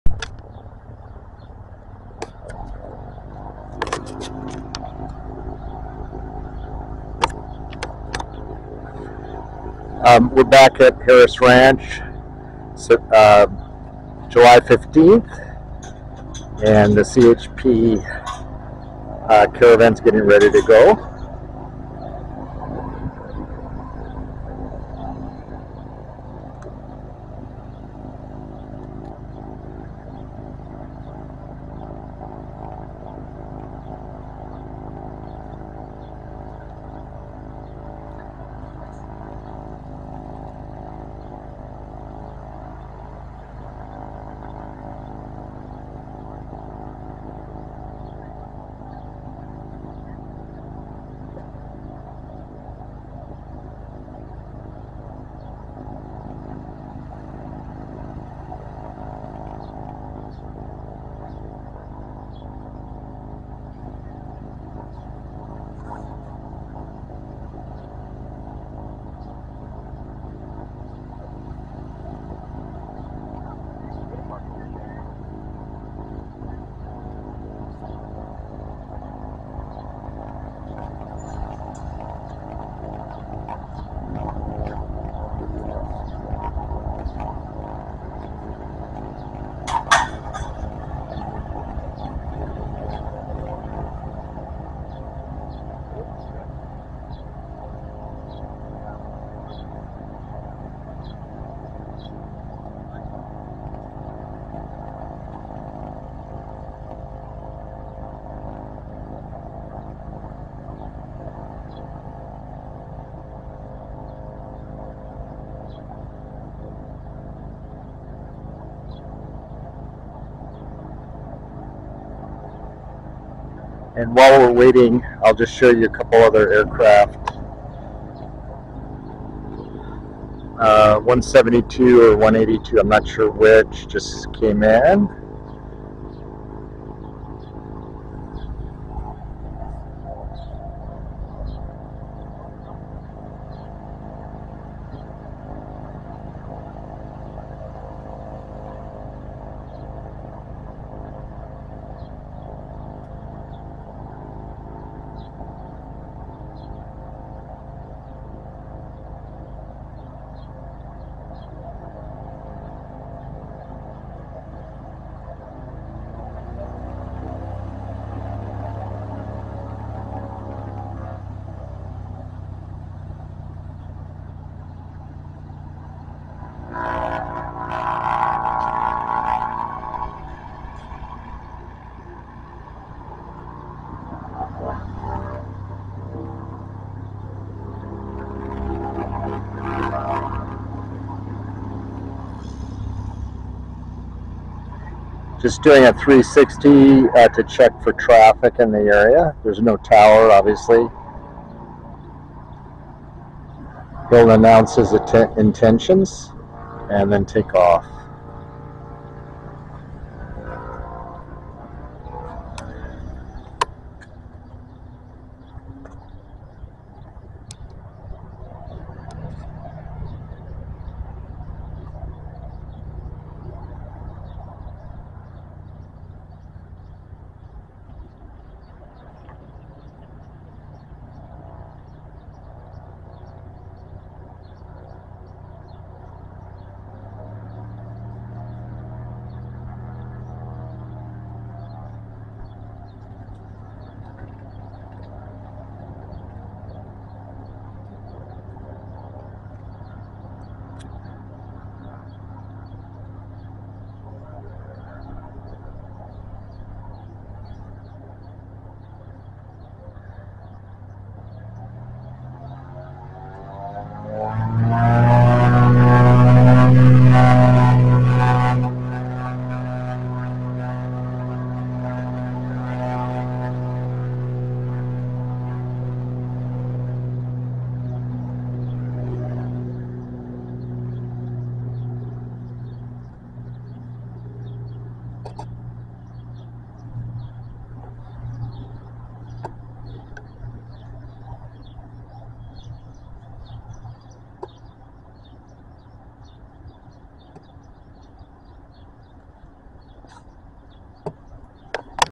CHP CARAVAN TAXI & TAKEOFF AT HARRIS RANCH COALINGA CA 7/15/23
TODAY (7/15/23) TWO CHP AIRCRAFT LANDED AT HARRIS RANCH IN COALINGA CA WHERE THE RESORT OPERATES A SMALL AIRSTRIP. THIS VIDEO SHOW CESSNA CARAVAN POWERED BY PT6 TURBOPROP. A COMPANION VIDEO SHOWS CHP HELICOPTER WITH ALL SORTS OF SPECIALIZED EQUIPMENT.